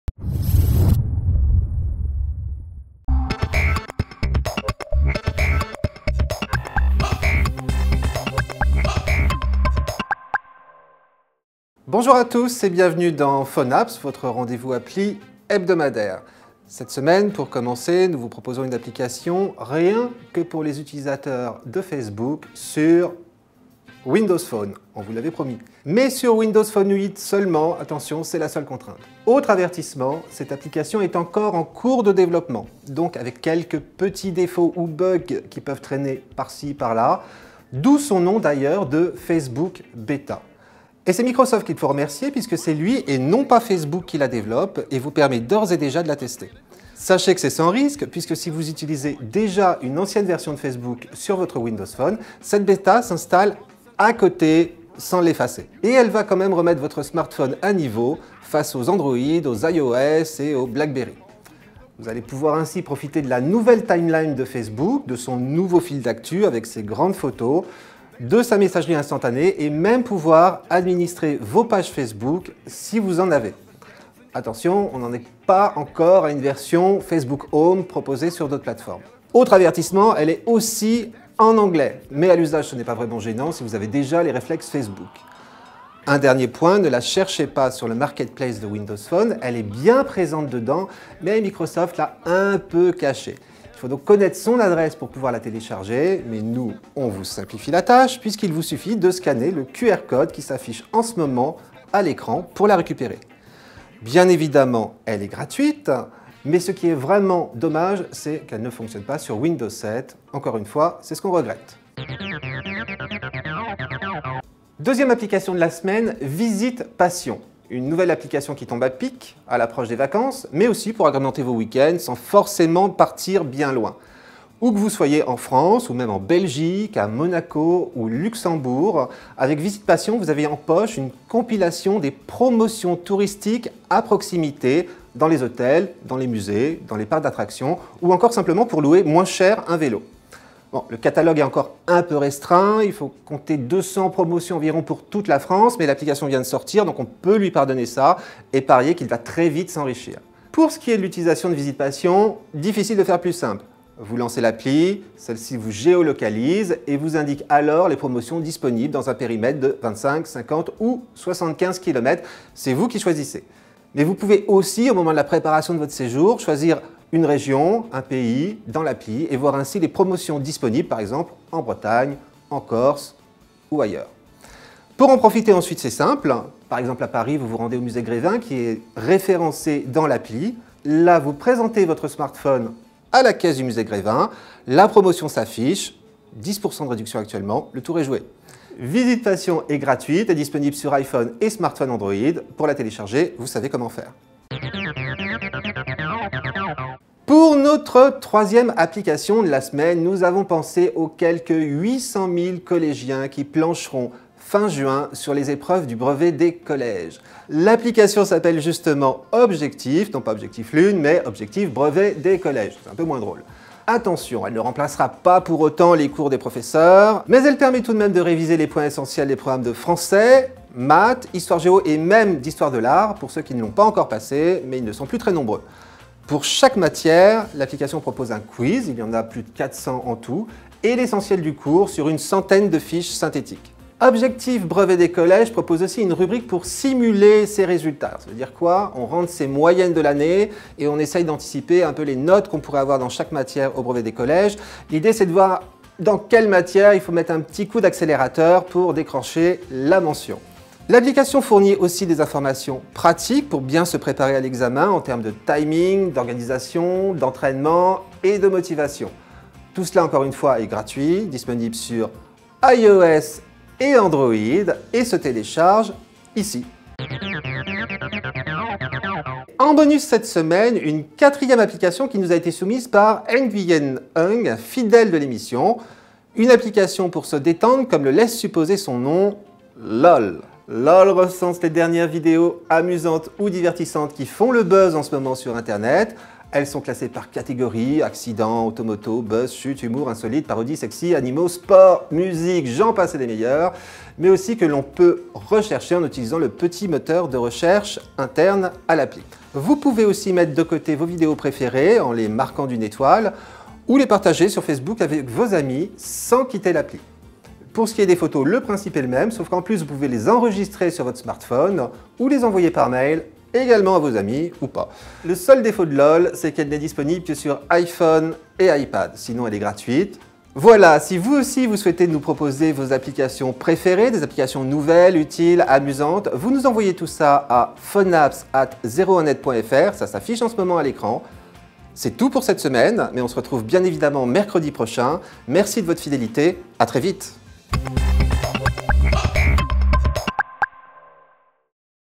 PhoneApps #07 : Facebook Beta, Visites Passion, Objectif Brevet des collèges, LOL (29/05)
Au sommaire de ce numéro :
- Facebook Beta pour Windows Phone 8
- Visites Passion : l'application qui tombe à pic pour vos vacances, sur iOS et Android
- Objectif Brevet des collèges : une appli pour aider les jeunes "3ème" pour le brevet sur iOS et Android
- LOL : appli pour se détendre et rire devant son smartphone (uniquement iOS)